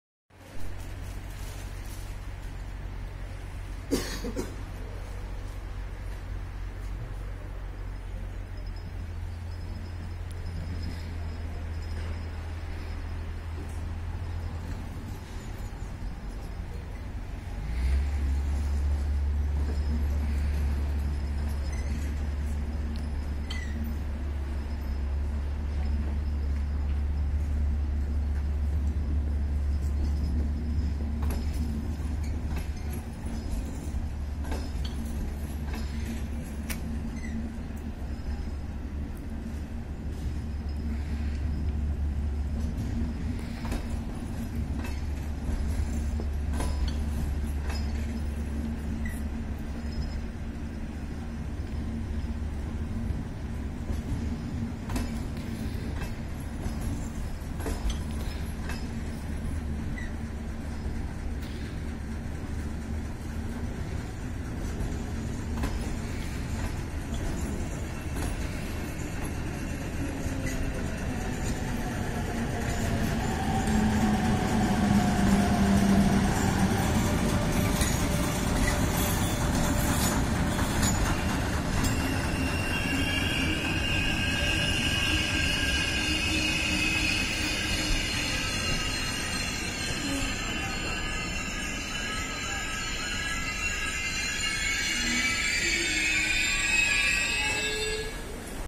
Tolatás KISS motorvonattal - Dunakeszi-Gyártelep
A Dunakeszi Járműjavító Dácsiája (A39,018) tolat egy KISS motorvonattal a gyártelepi állomás mellett.
A felvétel 2019. október 28. reggel, röviddel 8 óra után készült munkába menet telefonnal, így nem volt lehetőségem nagyon rákészülni.